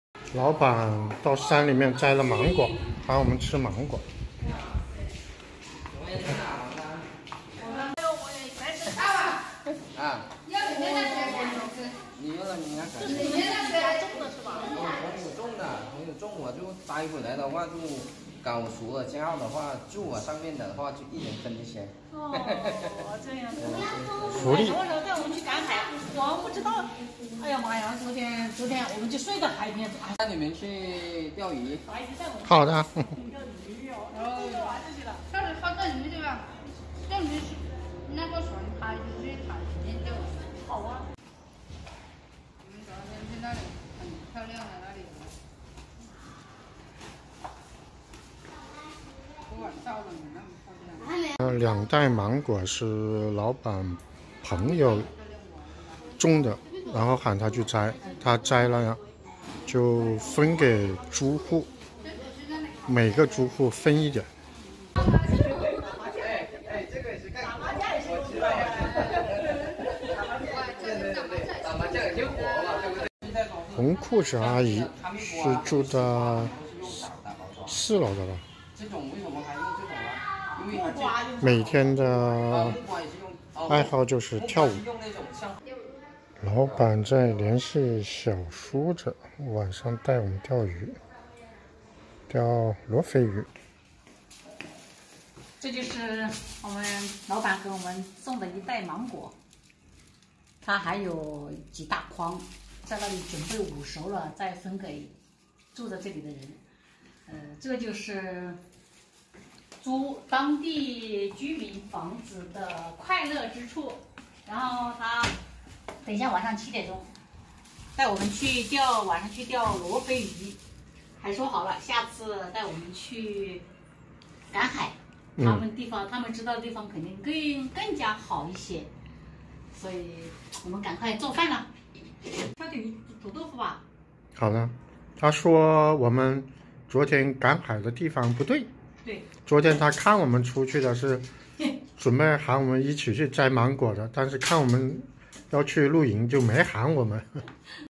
旅居海南，租住当地人房屋的好处，房东今晚带我出去玩
6070Vlog
优质vlog领域创作者
家有肥猫，其主好玩。一车两人，四海为家。